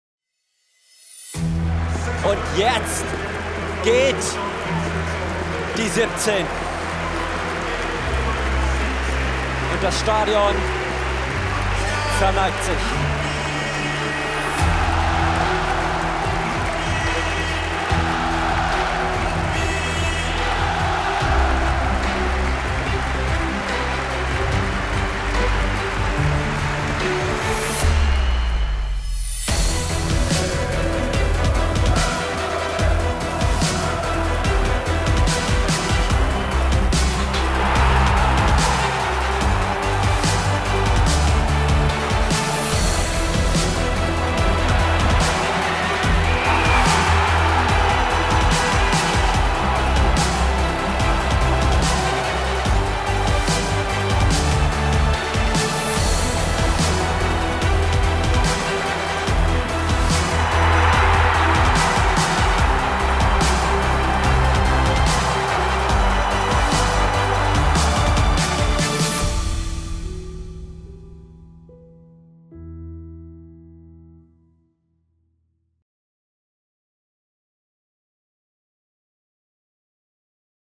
Danke, Lumpi!
Andreas "Lumpi" Lambertz beendet seine aktive Spielerkarriere und erweitert das Trainerteam von Fortunas U23. Danke für alles, Lumpi!

Fortuna Düsseldorf im Stadion hautnah erleben? Sichert euch jetzt Eure Tickets